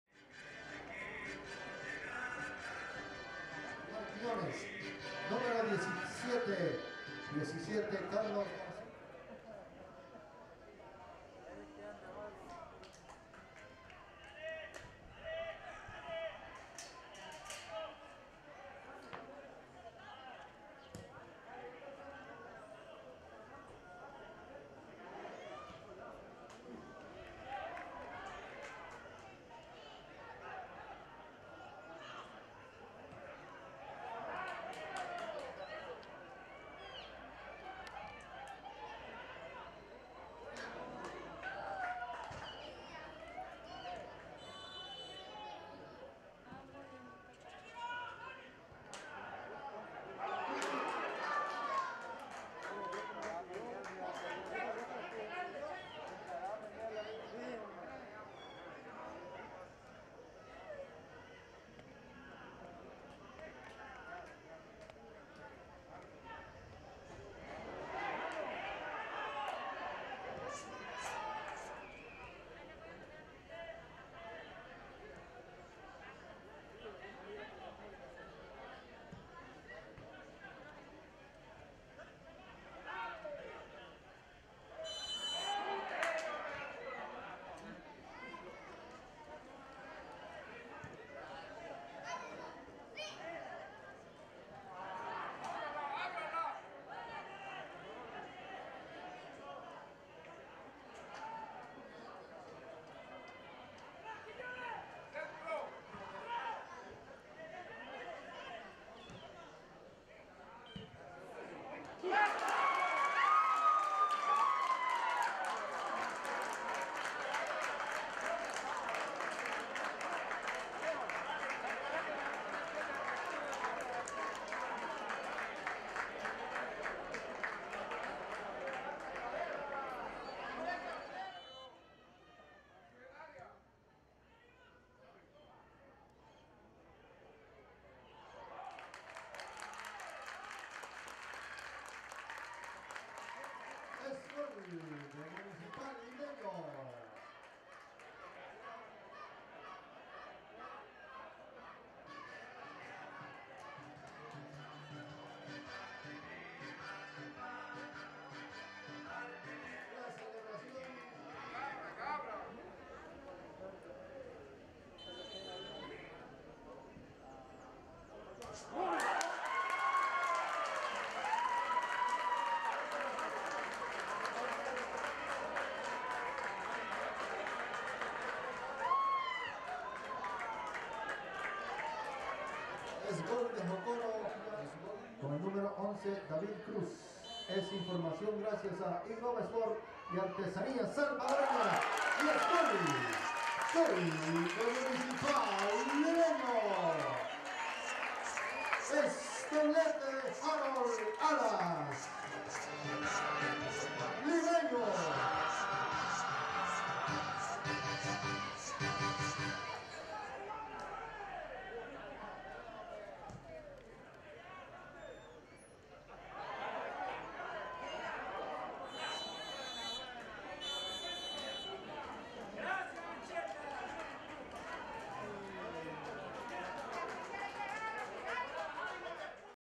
Resumen | Municipal Limeño 2 - 2 Jocoro | Grupo Oriente, Fase 1 Jornada 6| Clausura 2021
Emocionante encuentro del grupo Oriente

Goles:
Harold Alas 58´ 72´
Junior Padilla 49´
William Guerrero 64´